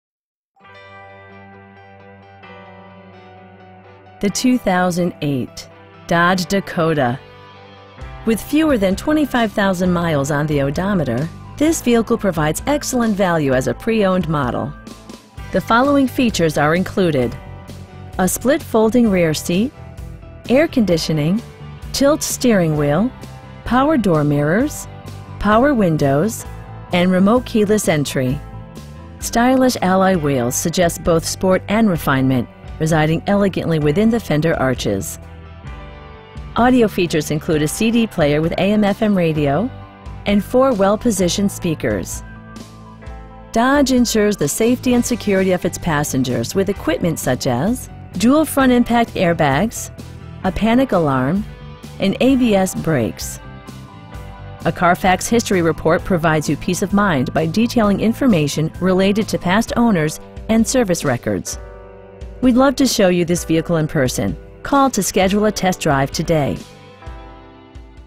2008 Dodge Dakota SXT/Bighorn/Lonestar Truck Short Crew Cab in Bryan, OH 43506
05768 State Route 15 North
Bryan, OH 43506

The 2008 Dodge Dakota. With fewer than 25,000 miles on the odometer, this vehicle provides excellent value as a pre-owned model. The following features are included: a split folding rear seat, air conditioning, tilt steering wheel, power door mirrors, power windows, and remote keyless entry. Stylish alloy wheels suggest both sport and refinement, residing elegantly within the fender arches. Audio features include a CD player with AM/FM radio, and 4 speakers, providing excellent sound throughout the cabin. Dodge ensures the safety and security of its passengers with equipment such as: Dual front impact airbags, a panic alarm, and ABS brakes. A Carfax history report provides you peace of mind by detailing information related to past owners and service records. We'd love to show you this vehicle in person.